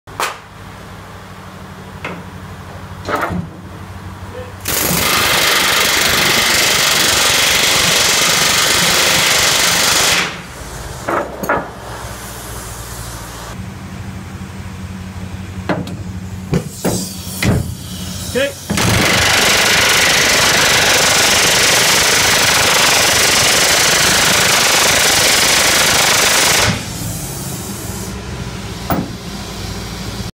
Hot Riveting Narrowboat Raven
Hot Riveting Grand Union Star Class Narrowboat ,Raven' formerly known as 'Hydrus'